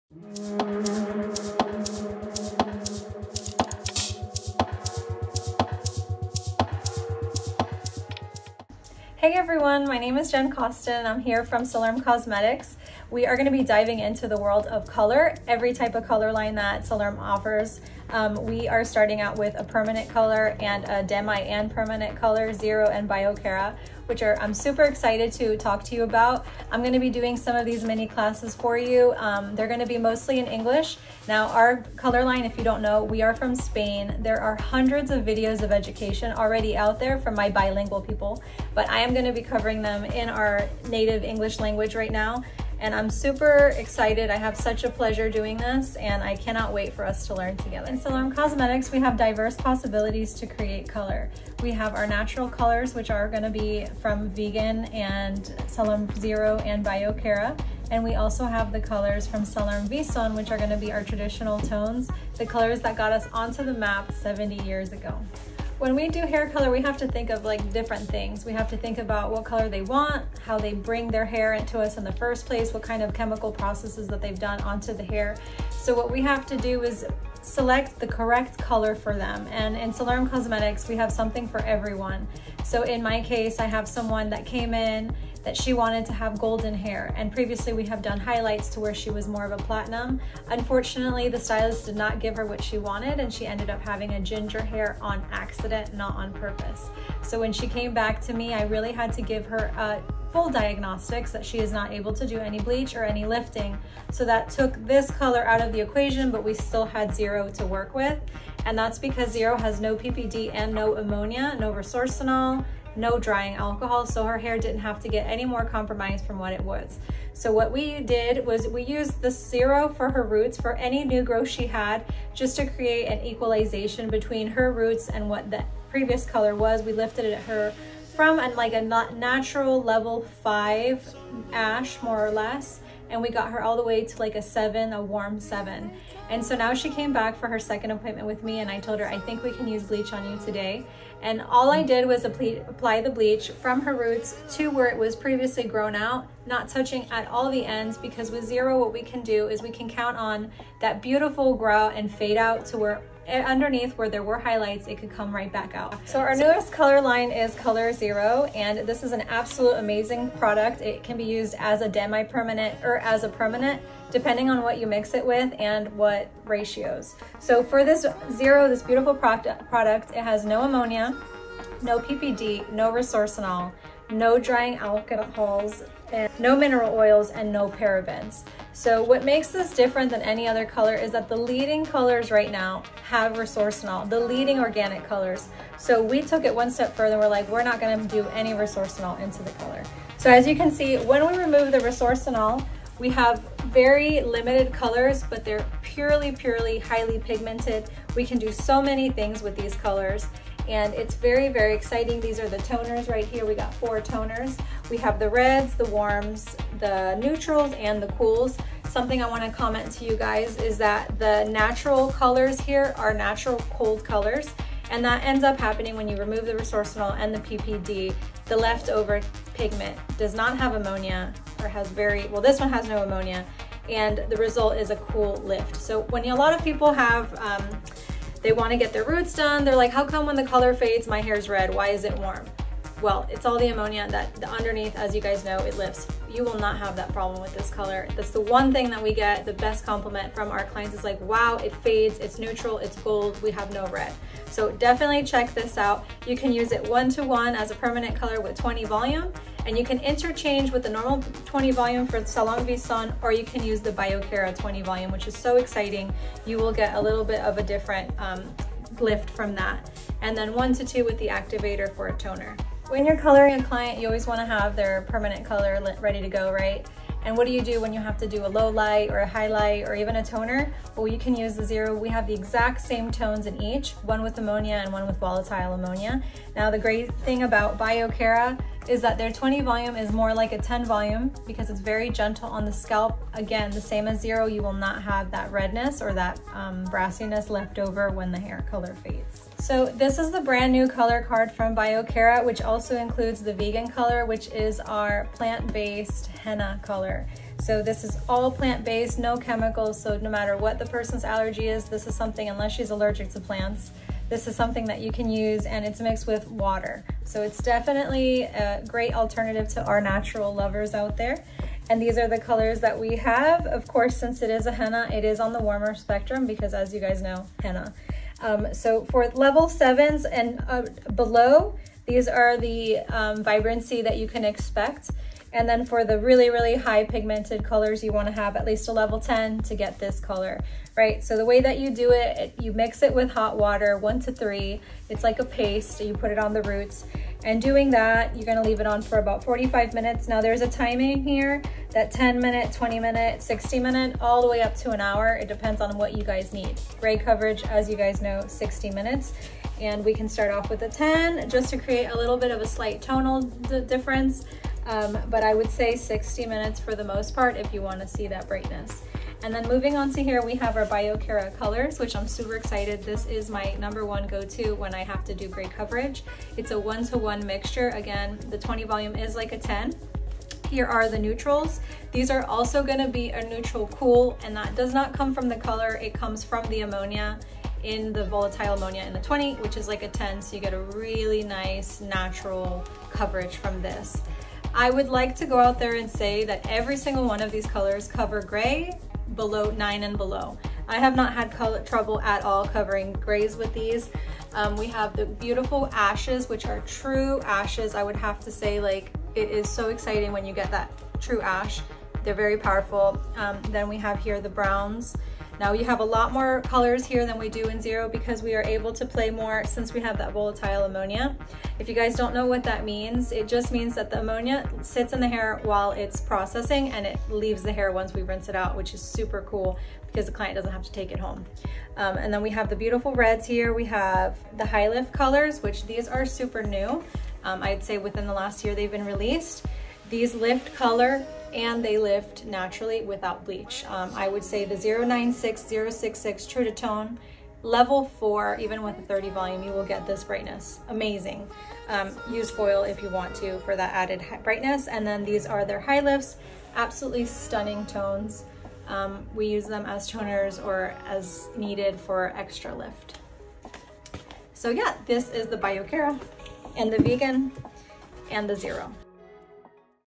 Discover the Biokera Natura and Zero best professional hair color lines | Salerm Cosmetics
Learn more about two of Salerm Cosmetics' most important hair color lines: Biokera Natura and Zero, with stylist Jeanne Coston. Discover the range of shades, application methods and how to achieve the best results.

Biokera Natura Color is a permanent oxidation dye with organic plant oils that helps to protect the hair fiber and keep color vibrant for longer.

Zero, permanent or demi-permanent color, your choice. Ammonia-free hair color with L-Arginine and DMG that offers optimum results, while treating and protecting the hair and scalp thanks to its oil-cream formula.